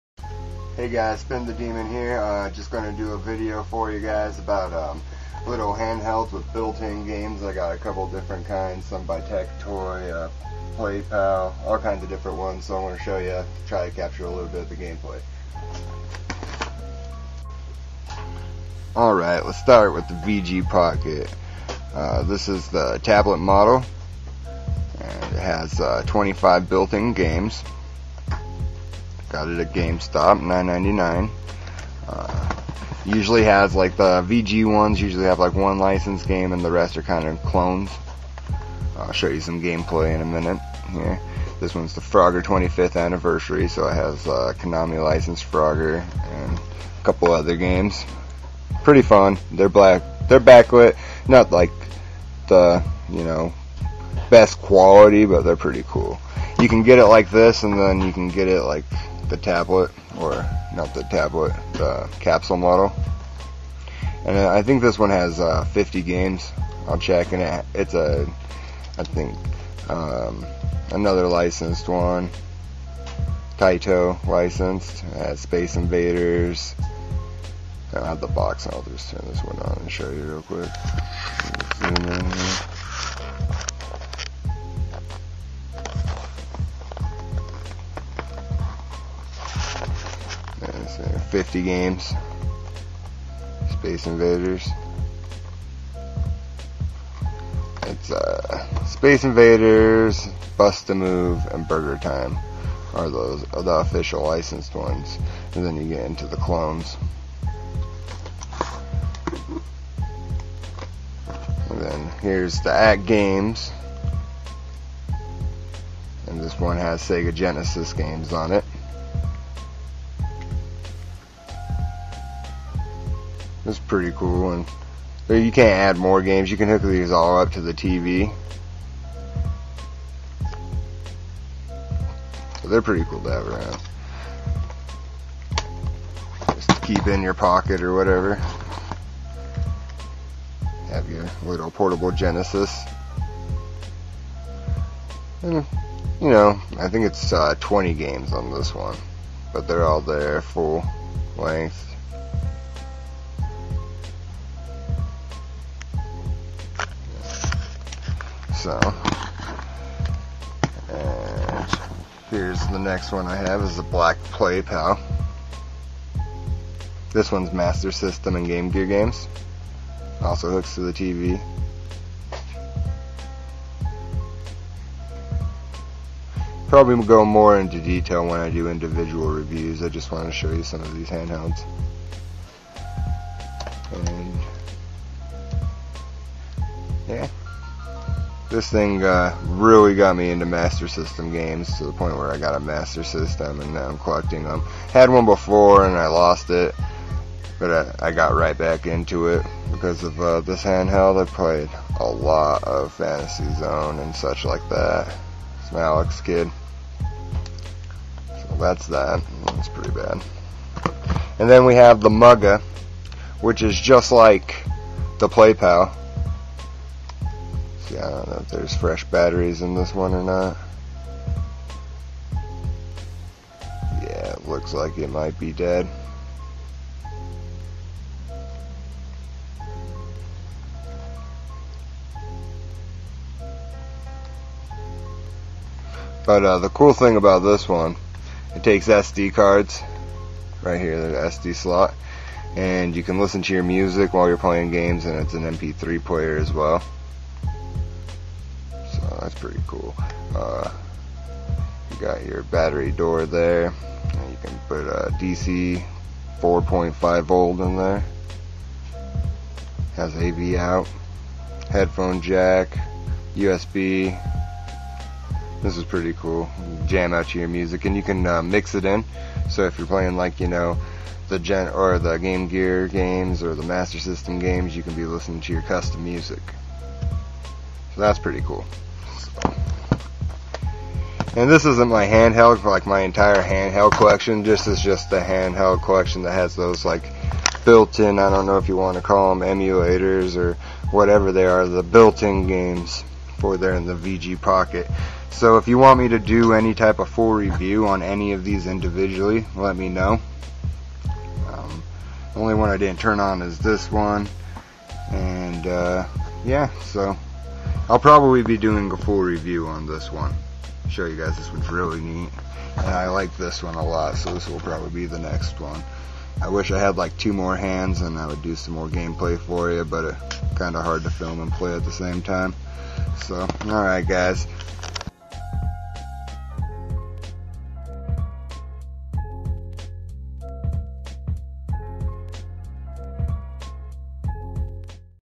5 Handheld With Built in Games
I show 5 random handhelds from my collection with built in games. All of them have screen protectors so thats why the screen on some look dirty or have a blur.

VG Pocket Tablet (Green)
VG Pocket Capsule  (Oranage)
ATGAMES Tec-Toy (Red and Black)
Play Pal (Black)
Muga gamep3 player White W/ Black